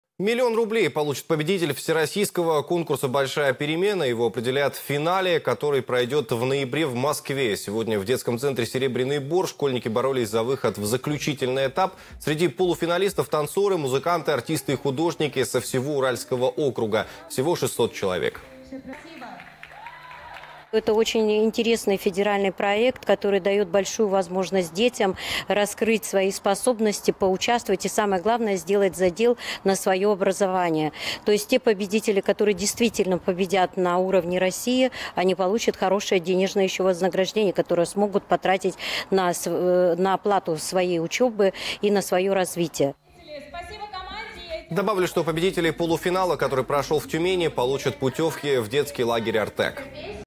За миллион рублей борются полуфиналисты Всероссийского конкурса для школьников
Миллион рублей получит победитель Всероссийского конкурса «Большая перемена». Его определят в финале, который пройдёт в ноябре в Москве. Накануне в детском центре «Серебряный бор» школьники боролись за выход в заключительный этап. Среди полуфиналистов танцоры, музыканты, артисты и художники со всего Уральского округа. Всего 600 человек. Победители полуфинала, который прошёл в Тюмени, получат путёвки в детский лагерь «Артек».